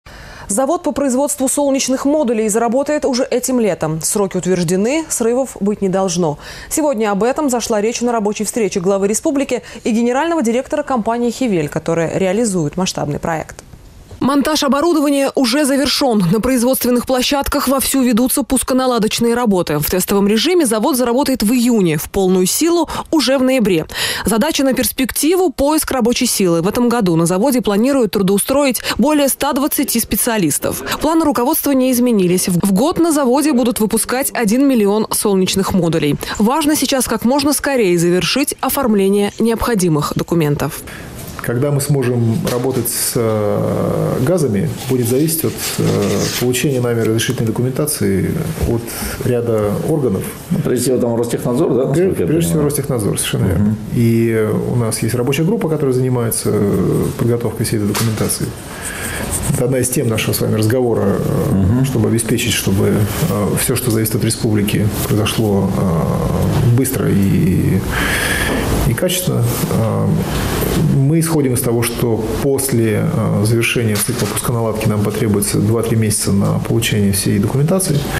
Завод "Хевел" заработает уже этим летом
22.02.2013
Сроки утверждены, срывов быть не должно. Сегодня об этом зашла речь на рабочей встрече главы республики и генерального директора компании «Хевел», которая реализует масштабный проект.